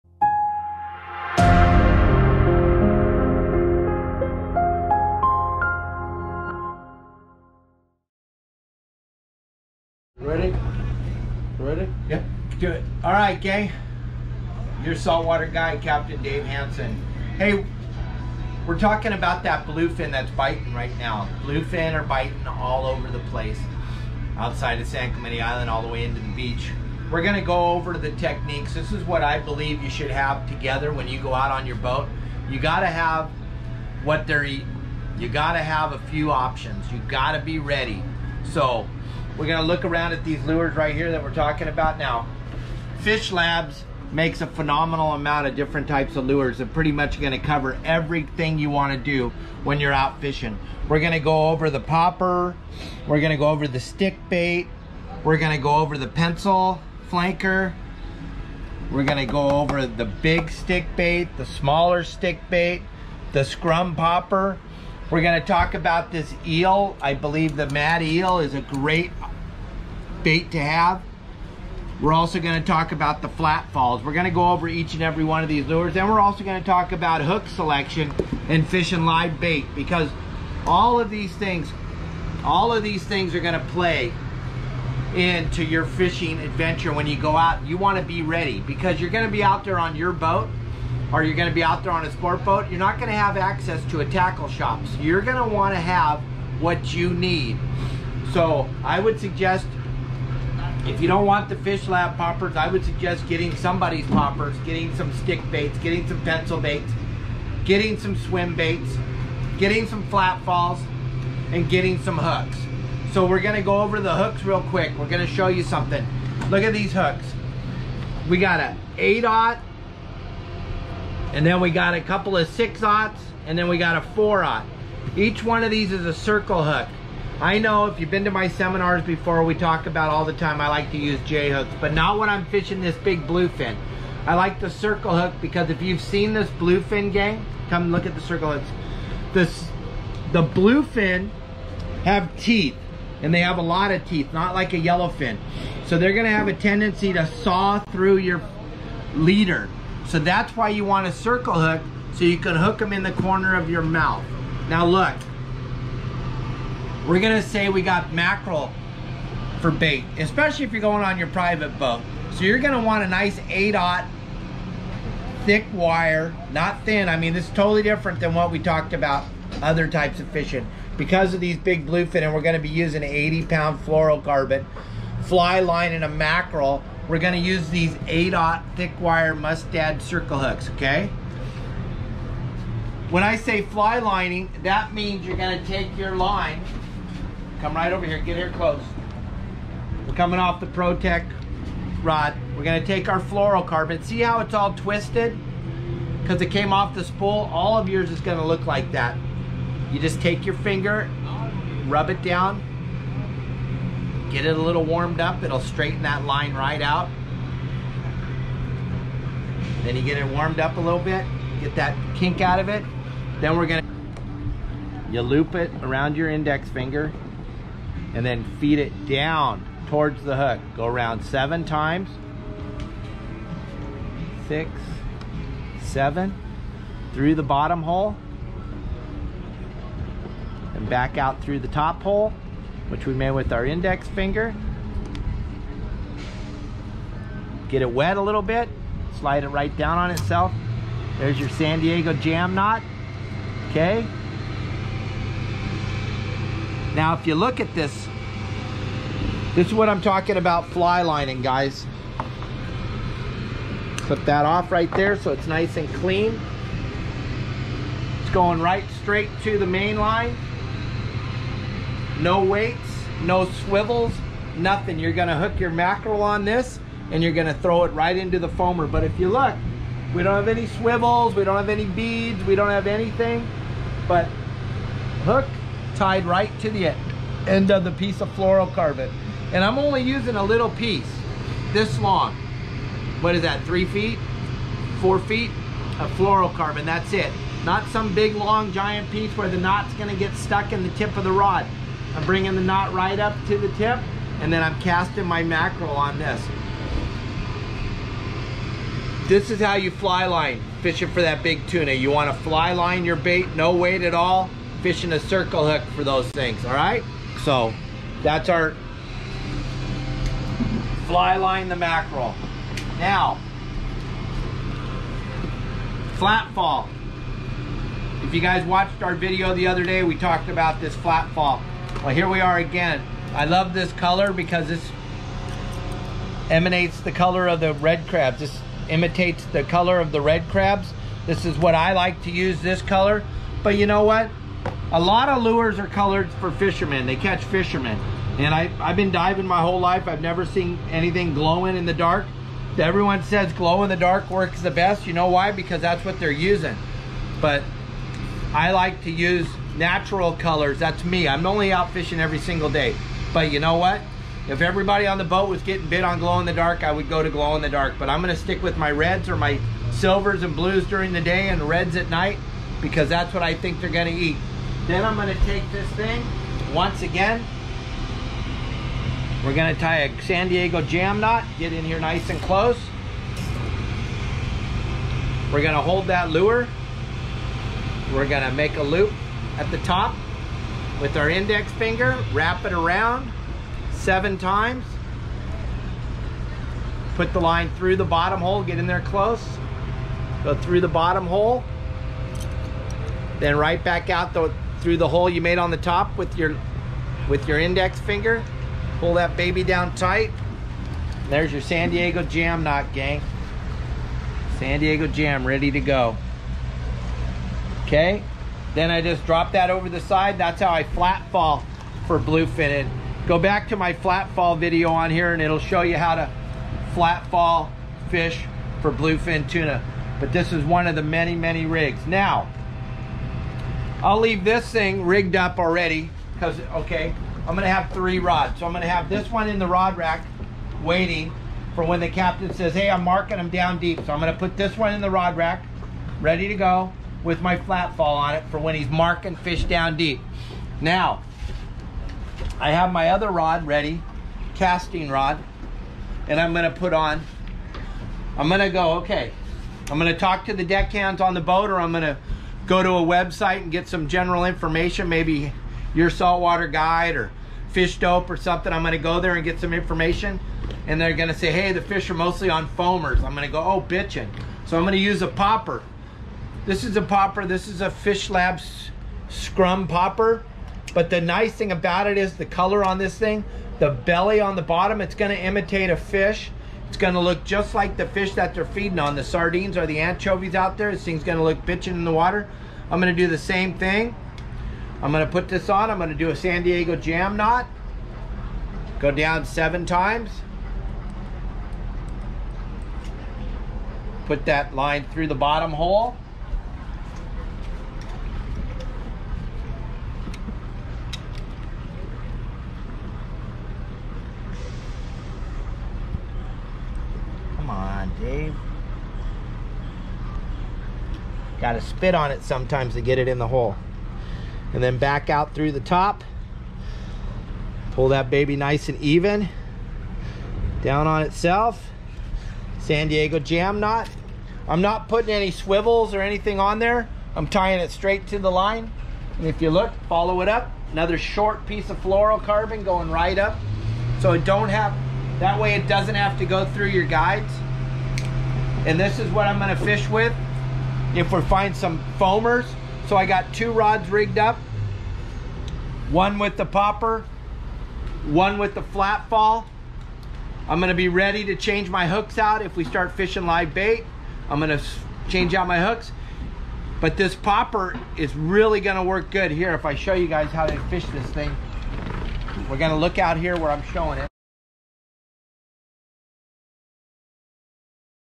Bluefin Tuna Tips | How to Catch Tuna (Gear, Tackle & Techniques!)
Captain Dave Hansen here, talking about How to catch Bluefin tuna. In this video you can learn everything from bluefin tuna gear, to hook selection, to even my recommended lures I like to use when fishing for bluefin tuna. Many anglers from all over the world, try their luck and or skill when fishing for these majestic species. I hope these bluefin tuna tips help those anglers land their fish of a life time.

Bluefin fishing is the pinnacle of Southern California sportfishing (and arguably the world) for its pure brute strength and incredibly tasty table fare.

Welcome to Your Salt Water Guide!
LEARN THE SECRETS OF SALT WATER FISHING

FROM EXPERT FISHERMAN CAPT. DAVE HANSEN